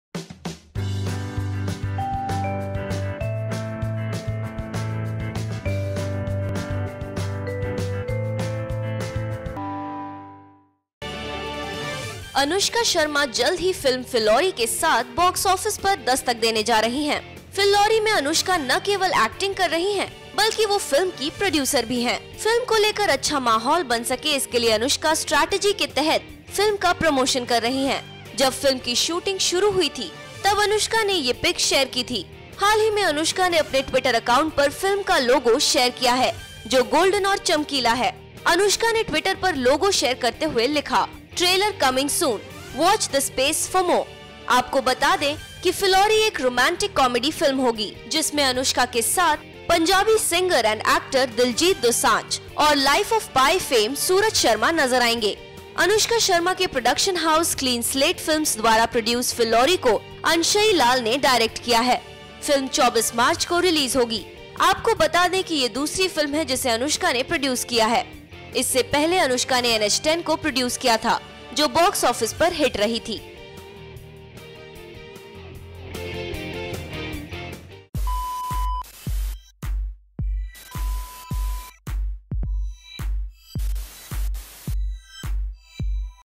Anushka Sharma and Diljit Dosanjh की Film Phillauri का First look हुआ Release
Anushka Sharma and Diljit Dosanjh are teaming up for the first time in Phillauri. This will be the actress' second production venture after NH10.